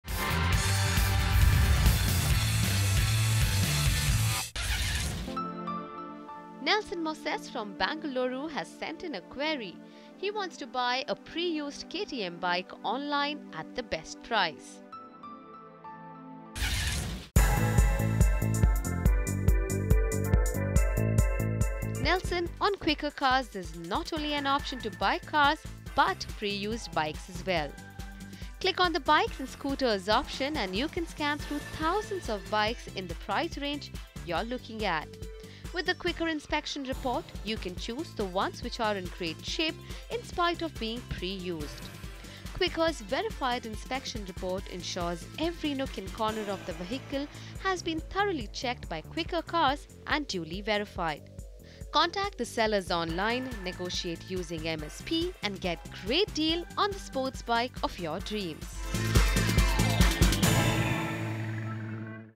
Buy high end bikes on QuikrCars
On QuikrCars there is not only an option to buy cars but also buy pre-used bikes as well. Find a variety of bikes in your budget.